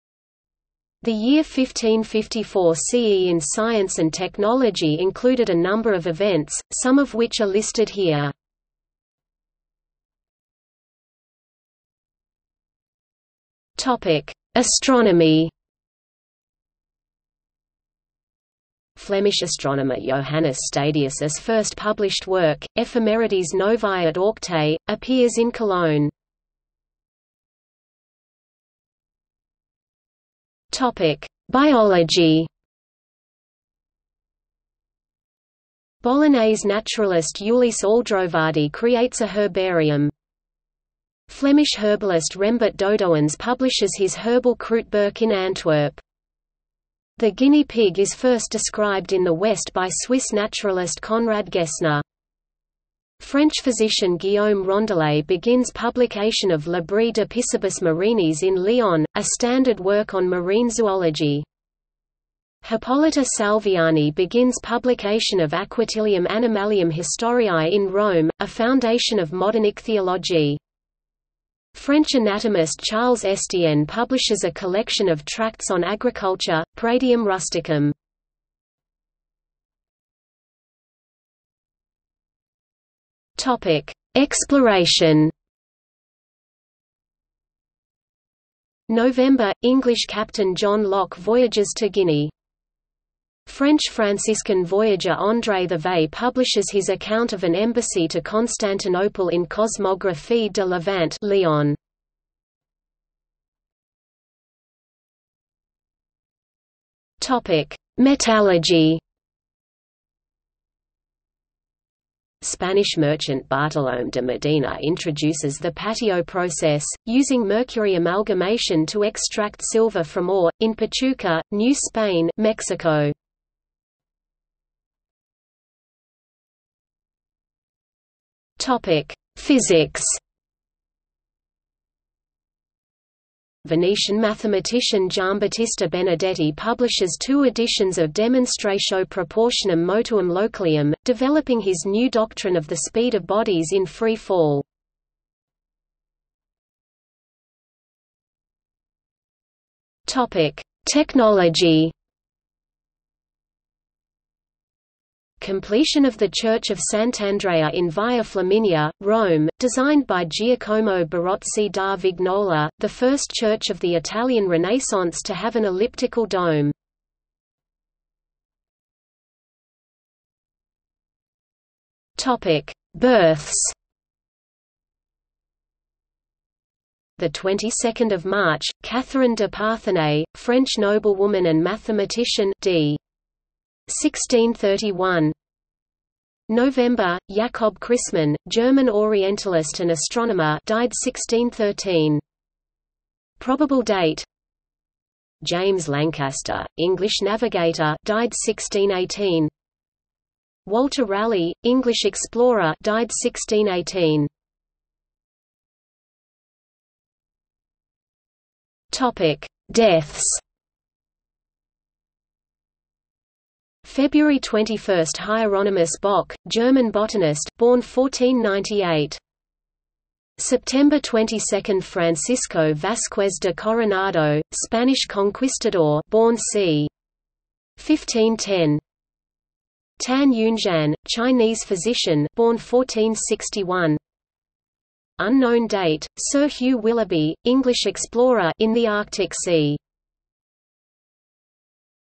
1554 in science | Wikipedia audio article
00:00:10 1 Astronomy
00:00:27 2 Biology
00:01:19 3 Exploration
00:01:43 4 Metallurgy
00:02:05 5 Physics
00:02:26 6 Technology
00:02:48 7 Births
00:03:25 8 Deaths

- Socrates

SUMMARY
The year 1554 CE in science and technology included a number of events, some of which are listed here.